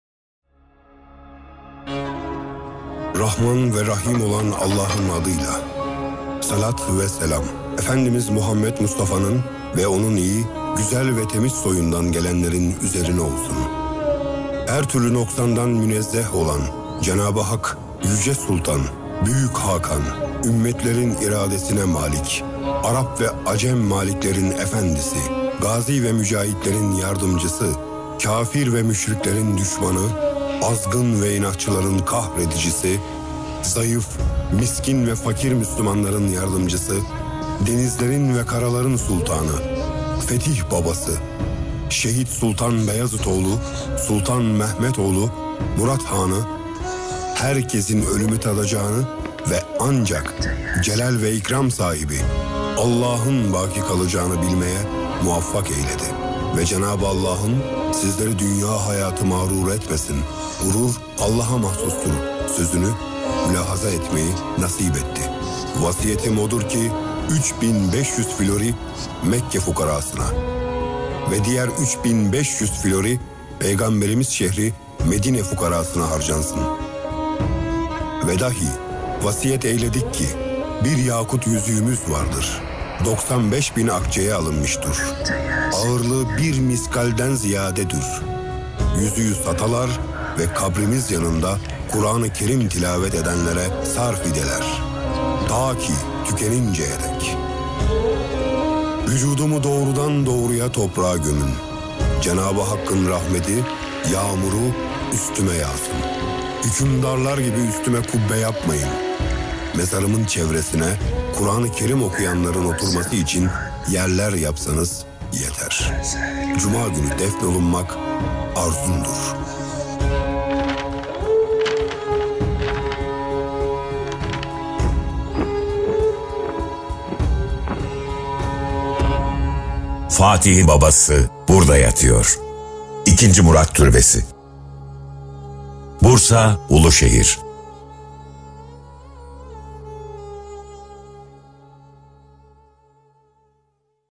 2nd Murat Shrine
2nd Murat Shrine
The father of Fatih the Conquerer sleeps here.
Mausoleum of Mourad, the second.